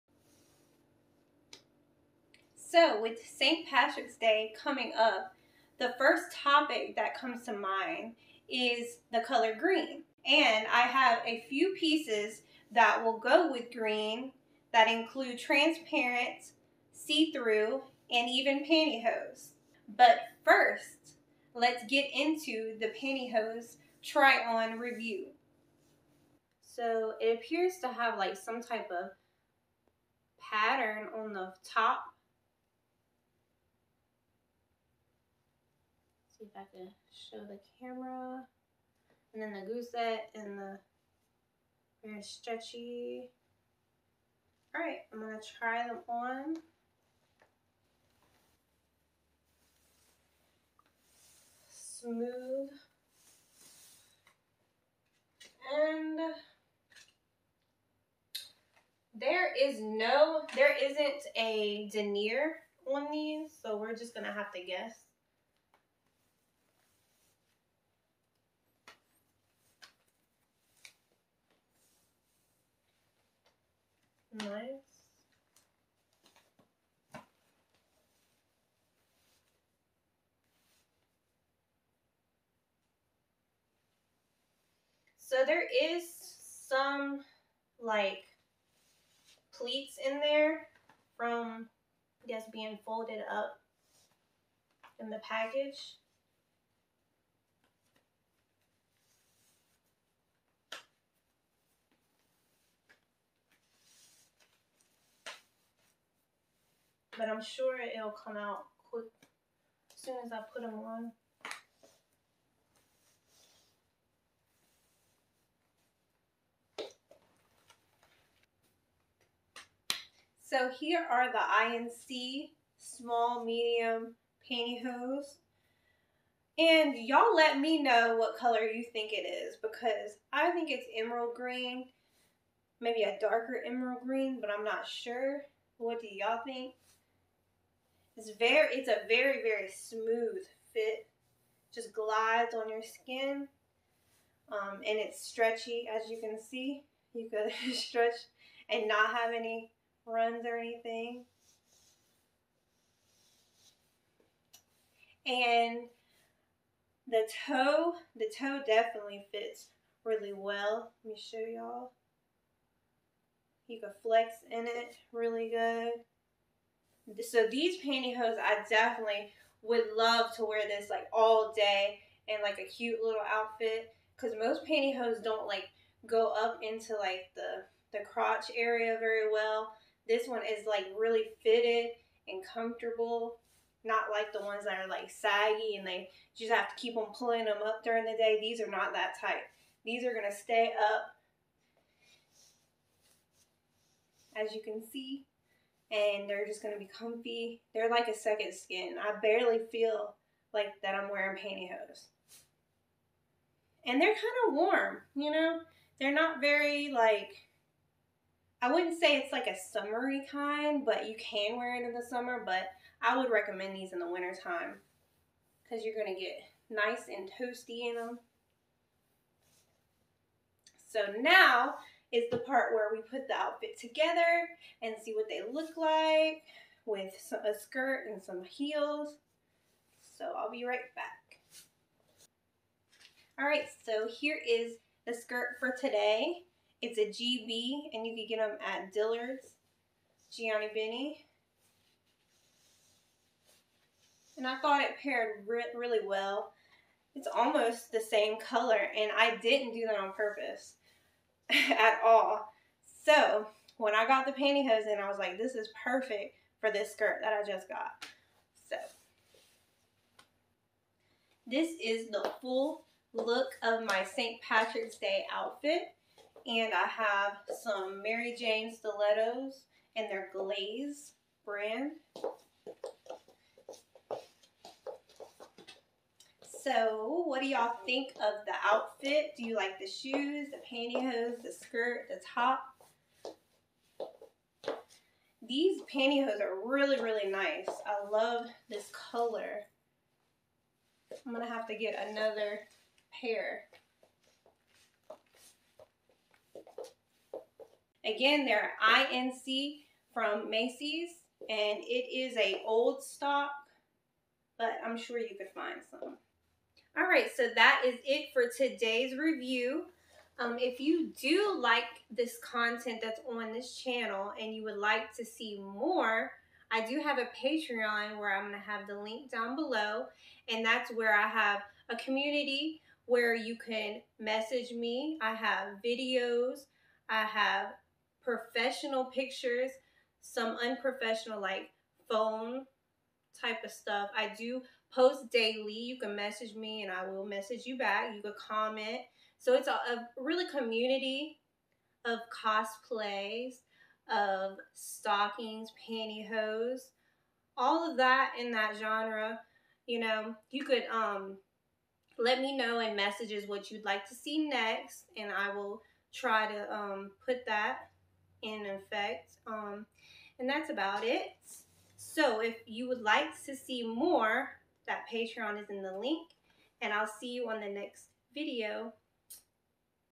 Unveiling Saint Patricks Day Pantyhose Try On in Mini Skirt
We begin the 4K video with a short intro, where I give thoughts for this try on haul and what you will see. The first try on is a pair of pantyhose. The mini skirt is from a GB and is Green in color. The try on is both sheer and opaque. I'll talk about the nylon material, shape, and how I would style with a mini skirt. Next I'll discuss the legs and how well they allow movement. This pair of pantyhose has a gusset in the middle. I'll then model the shiny skirt  last to complete haul together. Plus how well this look transforms your outfit with pantyhose fashion.